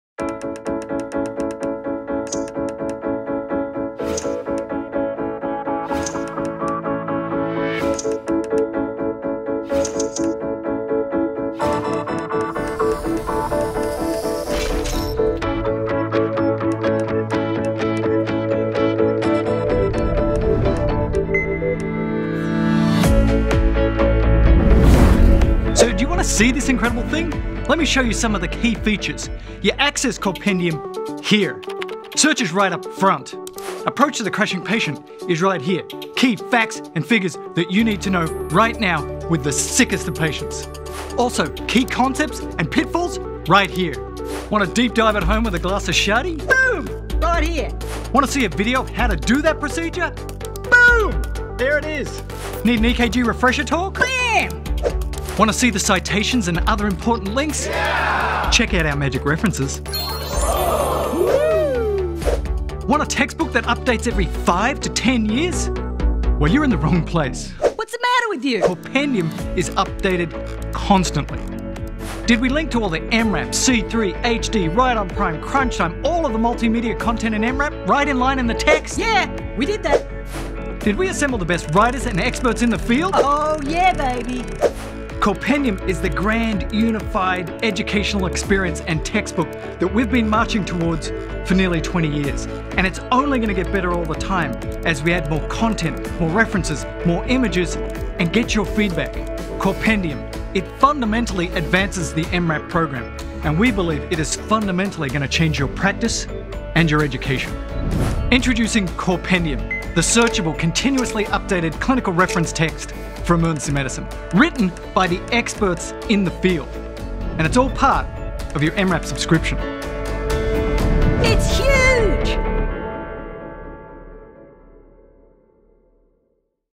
An Overview of Corependium
CorePendium is the revolutionary new online Emergency Medicine textbook by EM:RAP. Searchable, continuously updated, life-saving information. Available 24/7, in the palm of your hand. Mel explains some of its key features!

for all your Emergency Medicine education!